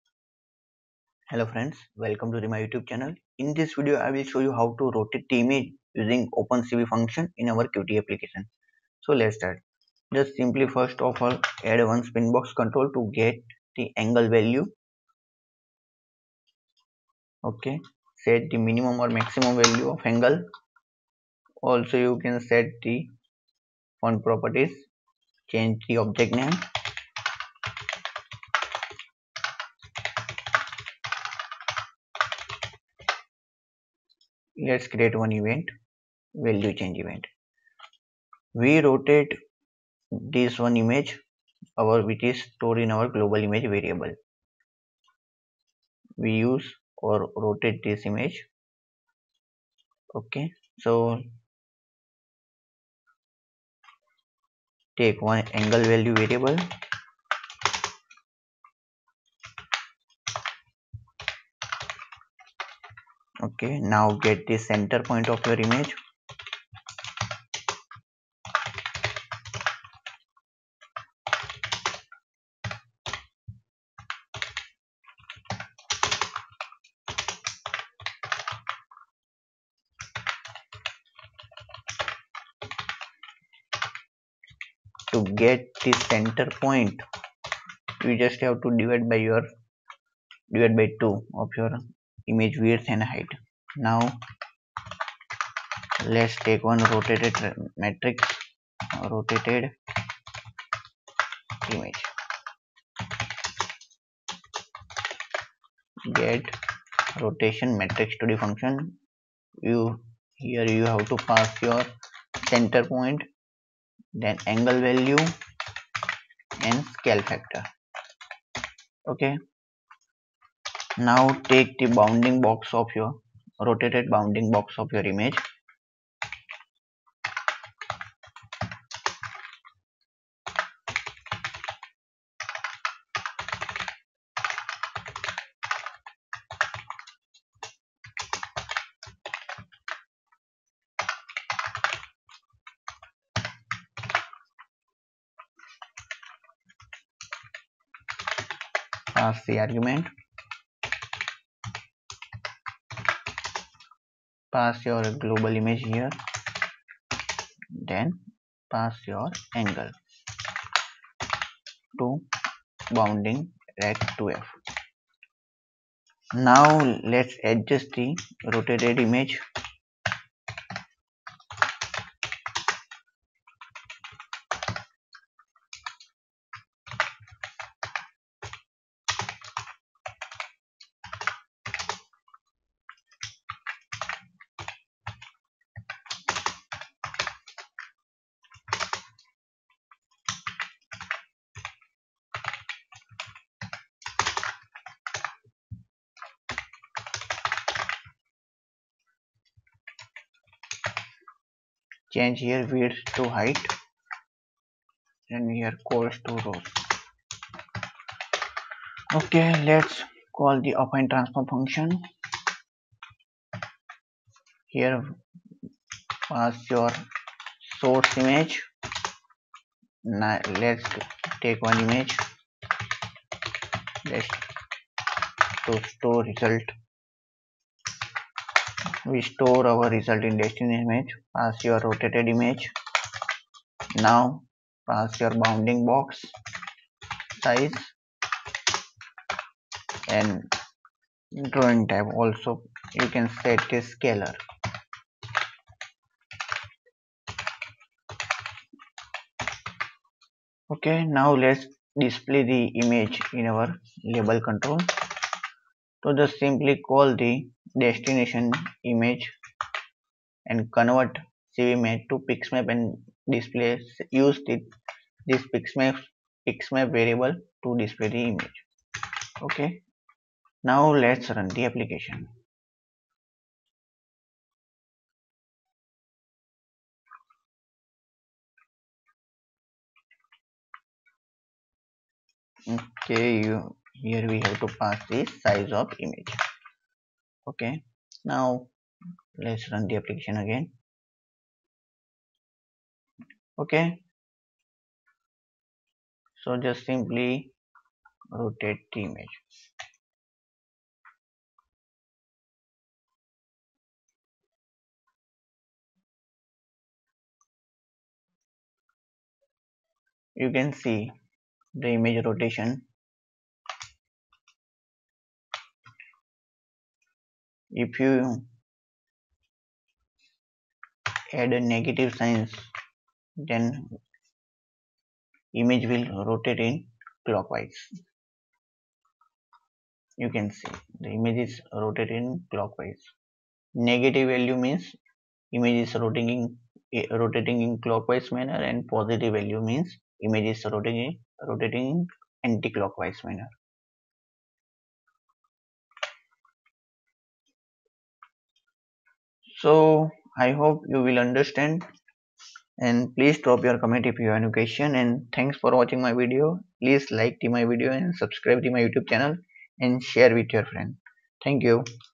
OpenCV | Image Rotation, Translation, Scaling | Computer Vision | Image Processing
Qt With C++ | Qt C++ | Qt Tutorial | Qt Designer Tutorial

In this tutorial, we will explore how to implement image rotation functionality using OpenCV C++ in a Qt application. OpenCV is a powerful computer vision library that provides various image processing functions, including rotation. By combining the capabilities of OpenCV with the flexibility of Qt, we can create a user-friendly application that allows users to rotate images with ease.

Throughout the video, we will guide you step-by-step on how to set up the development environment, import the necessary libraries, and write the code to implement the image rotation functionality. We will explain the concepts behind image rotation and demonstrate how to apply rotation transformations to images using OpenCV's functions.

By the end of this tutorial, you will have a solid understanding of how to use OpenCV C++ in a Qt application to implement image rotation functionality. Whether you are a beginner or an experienced developer, this video will provide you with valuable insights and practical knowledge to enhance your image processing skills.

Join us in this exciting journey of learning and start incorporating image rotation functionality into your own Qt applications today! OpenCV C++